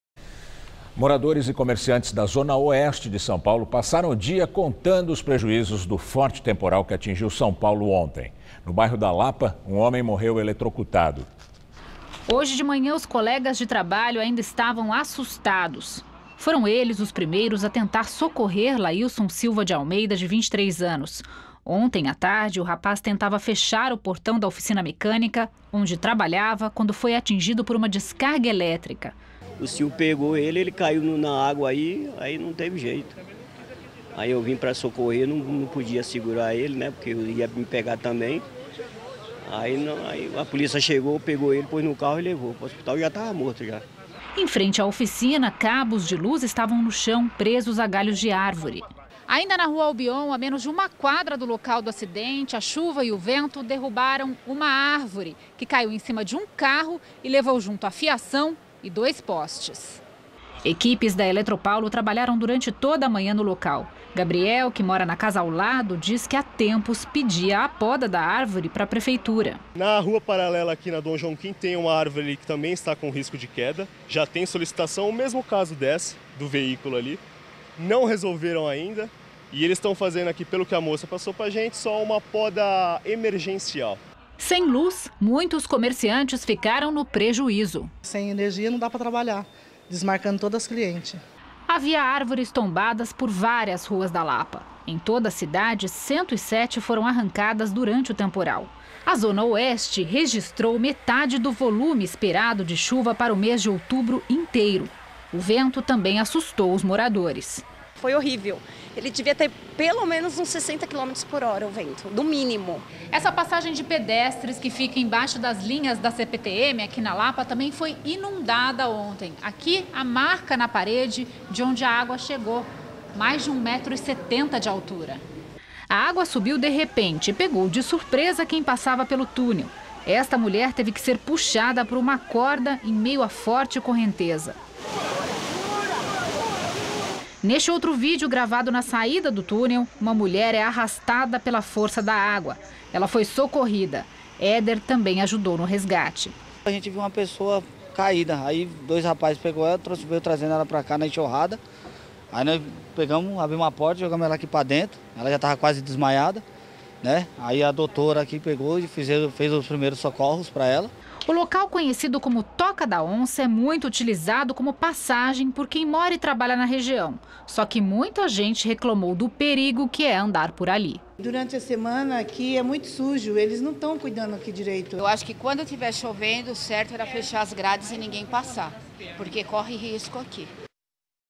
Após temporal, árvores tombadas e falta de luz
Moradores e comerciantes da zona oeste de São Paulo passaram o dia contabilizando os prejuízos do forte temporal que atingiu São Paulo ontem. No bairro da Lapa, um homem morreu eletrocutado.